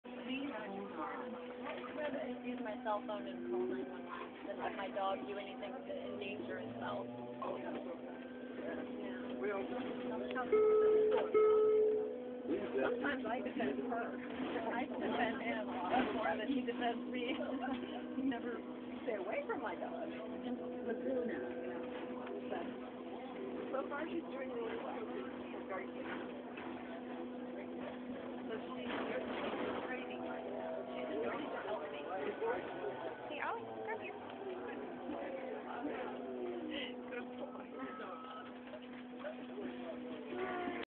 Tired service dogs on a bus
Ollie (Standard Poodle), Oliver (Italian Greyhound), and Iris (German Shepard) take the bus back after a visit to Golden Gate Park.